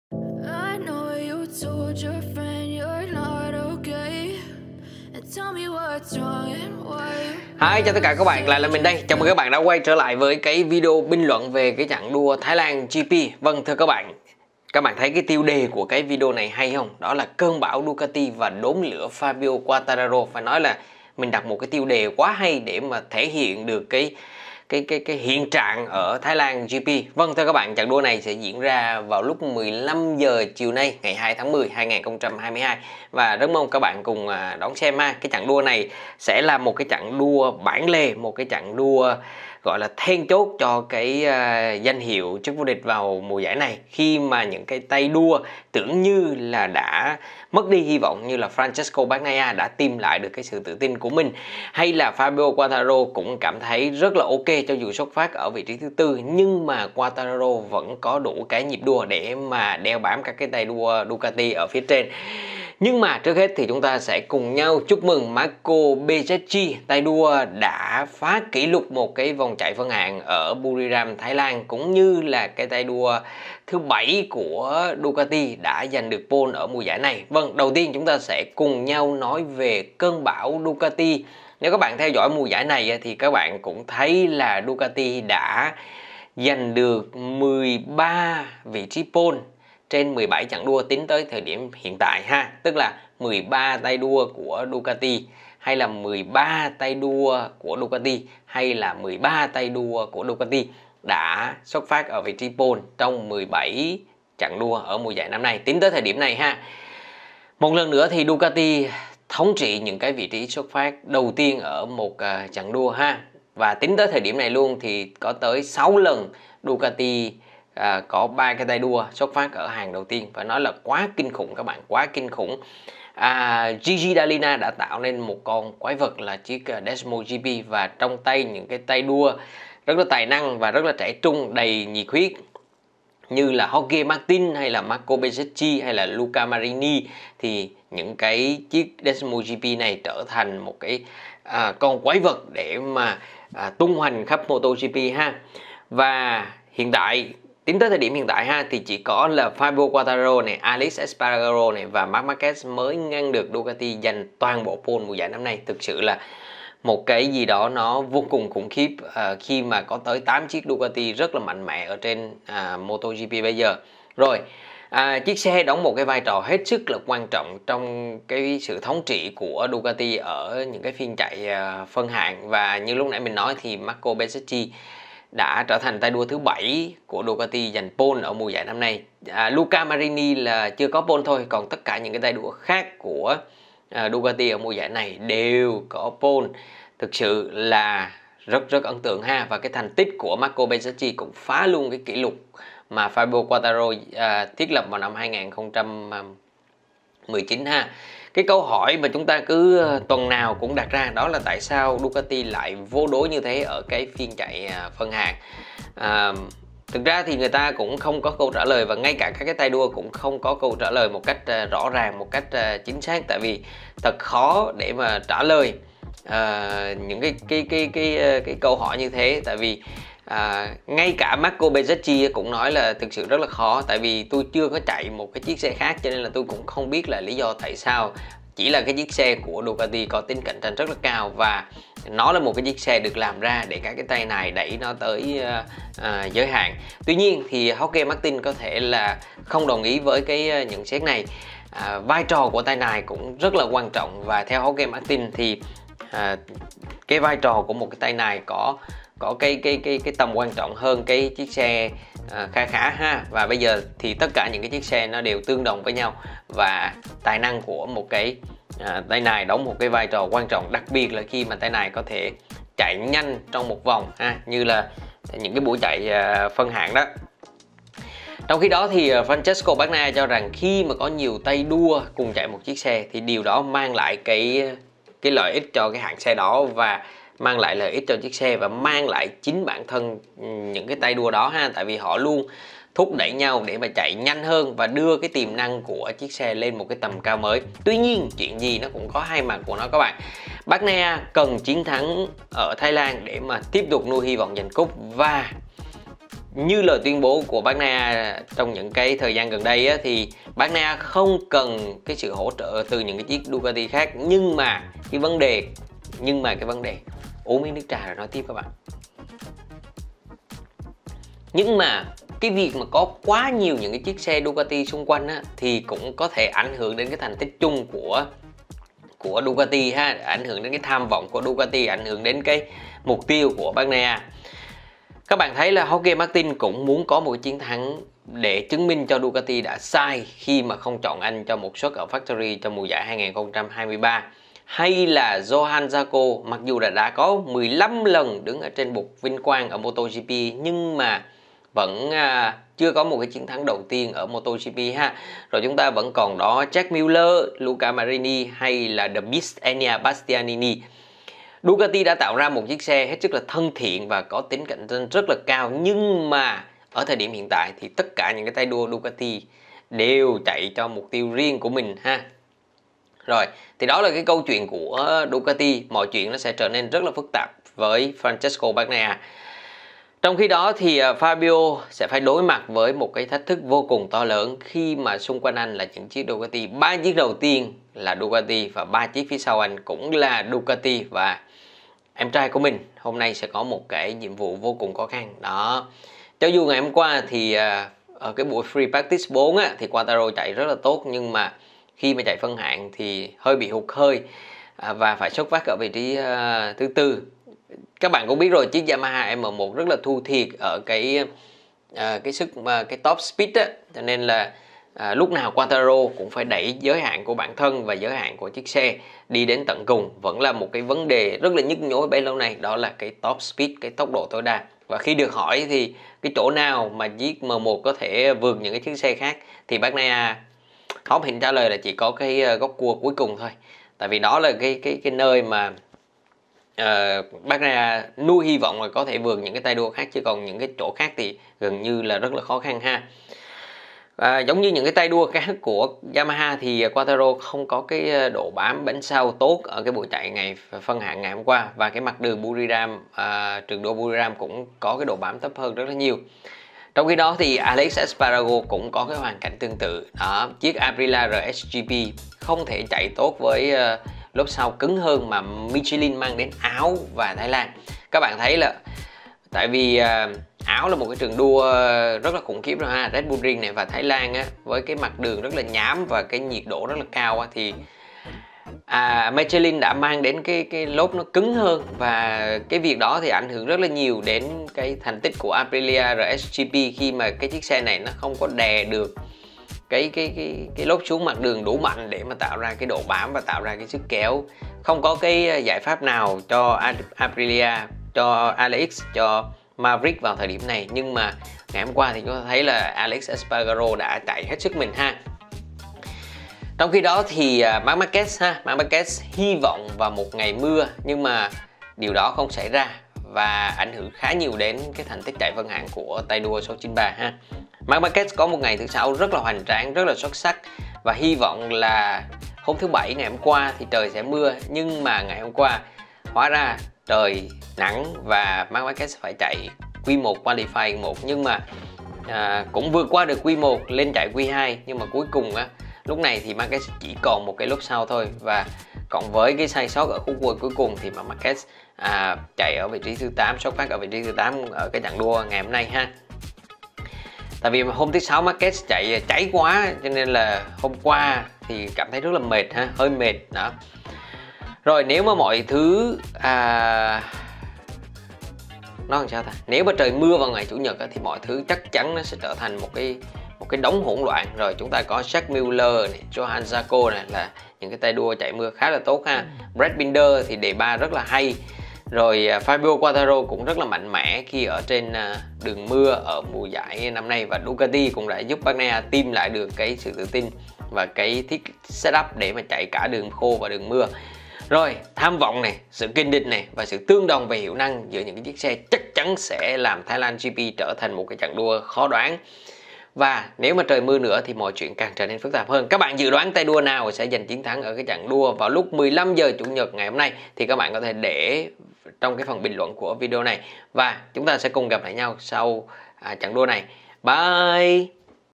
Đốm lửa Quartararo giữa cơn bão Ducati - Bình luận trước Thái Lan GP 2022
Tham vọng, kình địch, và sự tương đồng hiệu năng của những chiếc xe chắc chắn sẽ làm Thái Lan GP trở thành một chặng đua khó đoán. Thêm yếu tố mưa vào phương trình này, ai cũng có cơ hội để giành phần thưởng cao nhất.

Mua các sản phẩm khác ở link này để ủng hộ mình nhé

Tặng mình một ly cà phê nhé nếu bạn thấy video này hay:
Ly cà phê của bạn là nguồn động viên khích lệ đối với mình.